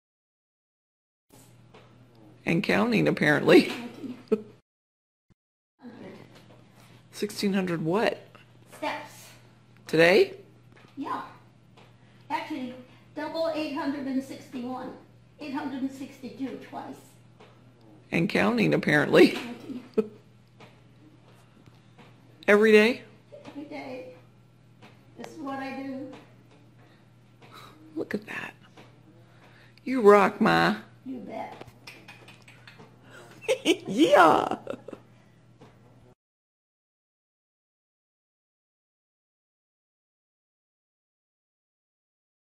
LOIS LIEBERMAN AGE 96 USES STAIRMASTER
860 STEPS AND COUNTING (every day)